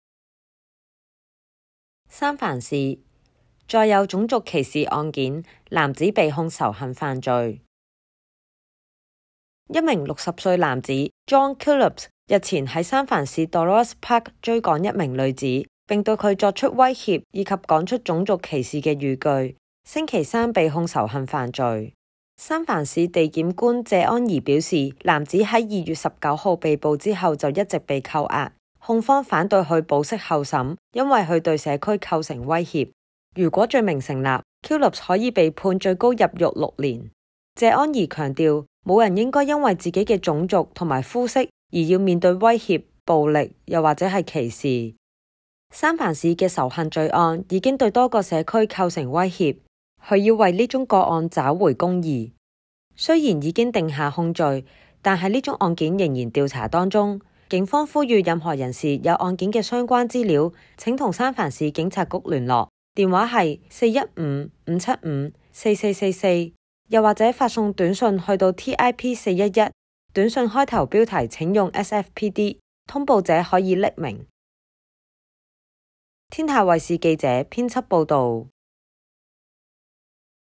【天下新聞】三藩市: 再有種族歧視案件 男子被控仇恨犯罪 SF man charged with hate crimes in yet another incident of racism
一名60歲男子John Killops
日前在三藩市Dolores Park追趕一名女子
並對她作出威脅以及説出種族歧視語句
星期三被控仇恨犯罪
三藩市地檢官謝安宜表示
男子在2月19日被捕之後就一直被扣押
控方反對他保釋候審
因爲他對社區構成威脅

如果罪名成立
Killops可被判最高入獄6年
謝安宜强調
沒有人應該因為自己的種族和膚色
而要面對威脅、暴力或者歧視
三藩市的仇視罪案
已經對多個社區構成威脅
她要為這宗個案找回公義

雖然已經定下控罪
但案件仍在調查當中
警方呼籲任何人士有案件相關資料
請與三藩市警察局聯絡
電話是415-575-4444
或發送短訊至TIP411
短訊開頭標題請用SFPD
通報者可以匿名